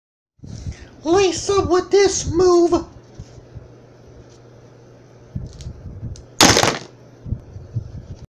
Lacey Evans & Her Super Lace Up Driver
Watch as The Former American Premier Wrestling World Heavyweight Champion does said move on her rival, off the scaffold and through 2 tables.